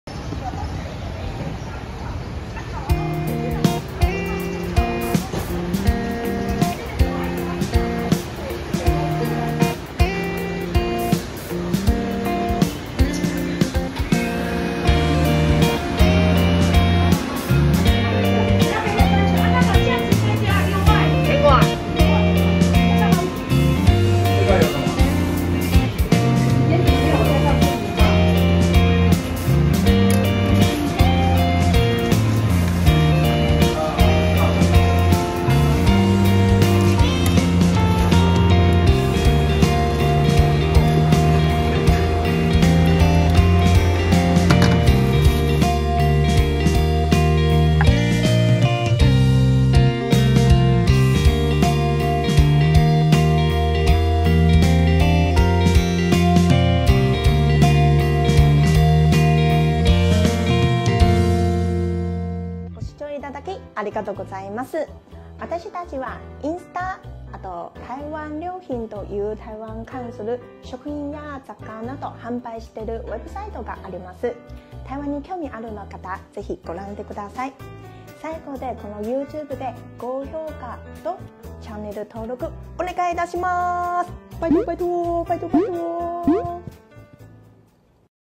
【今日の台湾グルメ#4】まったく新しい食感の麺、米苔目(ミータイムー)/永樂米苔目
永樂米苔目
住所：台北市大同區民樂街111號

【その他の関連動画はこちら】